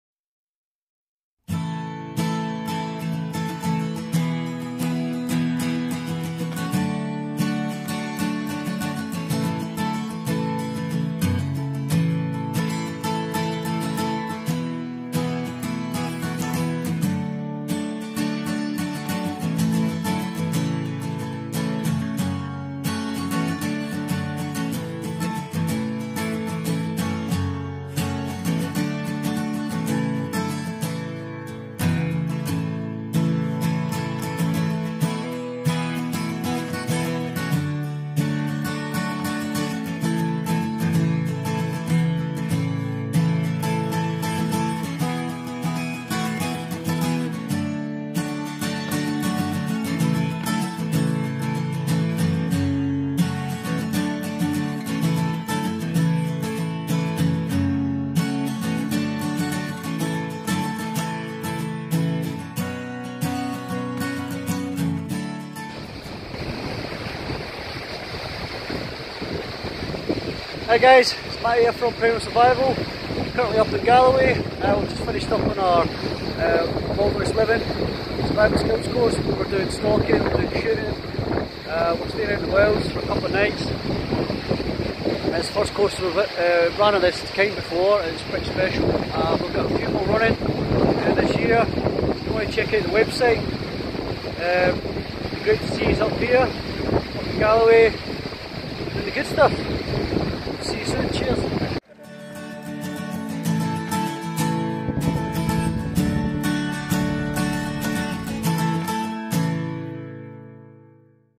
Primal Hunter Survival Skills Course. (Learn Deer stalking in Scotland.)
Want to learn how to stalk deer in Scotland?

Primal are delighted to have partnered up with South Ayrshire Stalking to deliver our new weekend wilderness living and hunter gatherer skills course.

Learn traditional wilderness living skills, tracking, ethical deer stalking, weapon, safety, marksmanship principles, foraging, fishing, traditional cooking methods, knife skills and lots more.

Places are limited to only 4 people per course.